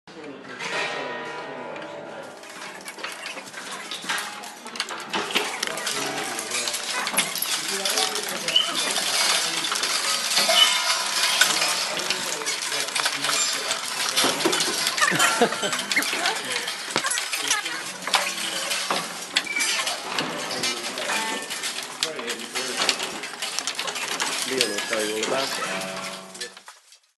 Dismantling Strings
The Pianodrome surgeons step into the Pianolab to dissect a specimen in order to better understand its inner workings and read the future in its guts...